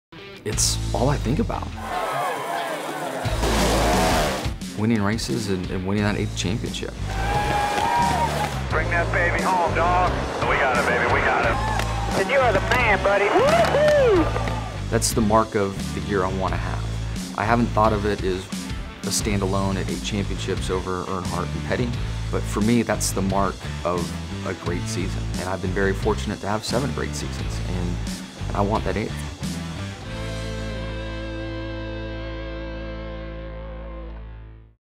Johnson: I want that eighth championship
Jimmie Johnson is on the hunt for an eighth championship to surpass Richard Petty and Dale Earnhardt in the NASCAR record books for the most titles in the sport's top series.

Play every weekend!